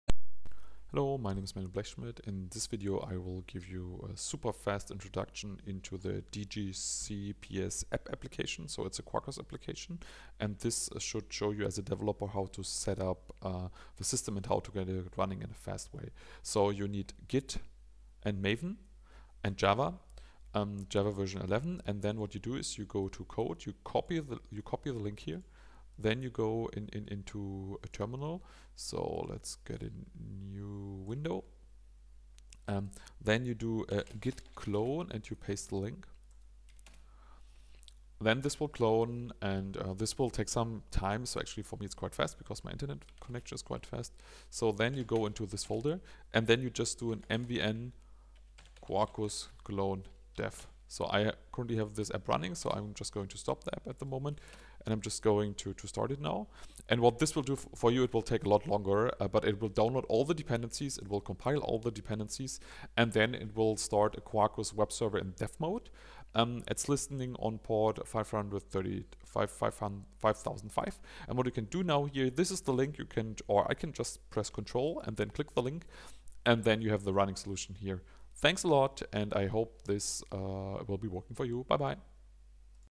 dgc-ps-app developer set up
This video shows how to set up the dgc app using mvn, java and quarkus.